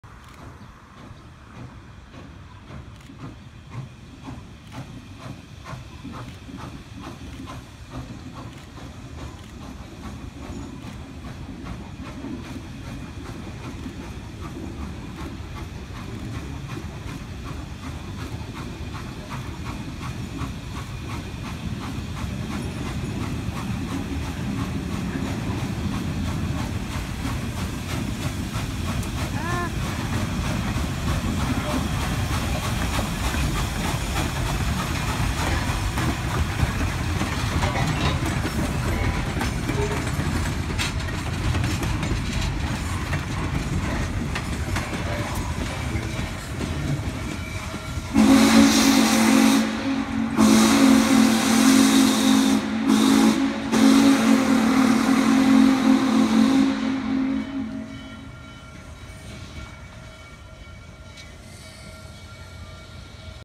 J 611 Spencer NC NCMT July 3, 2016 v
Norfolk and Western steam locomotive J-611 making "Behind the Throttle" run on premises; North Carolina Museum of Transportation. July 3, 2016.